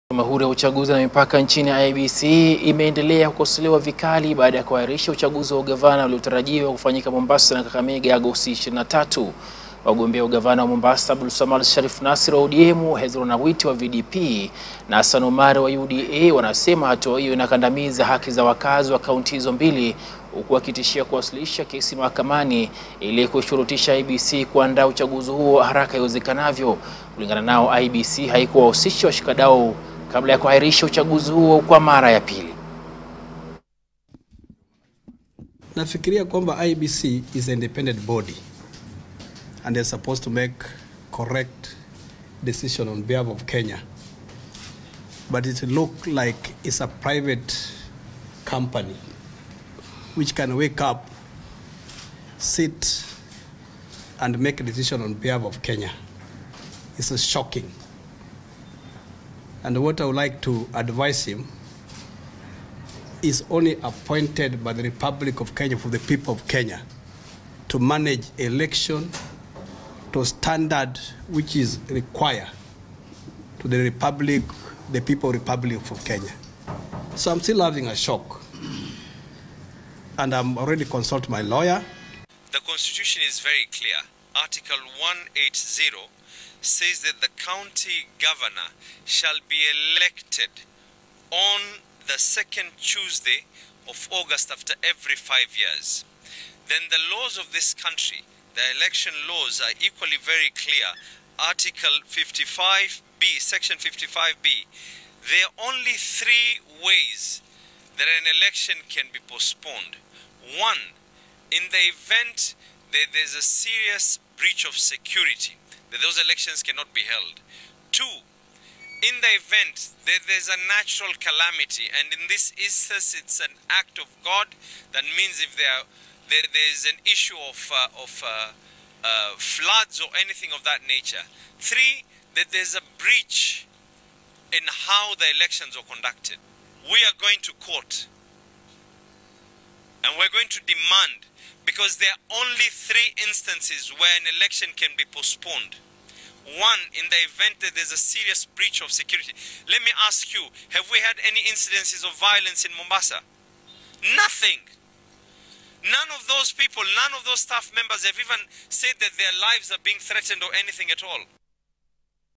Hasira za kura ya ugavana Mombasa zaendelea
dirayahopetv naye Dickens Luvanda
Follow us on: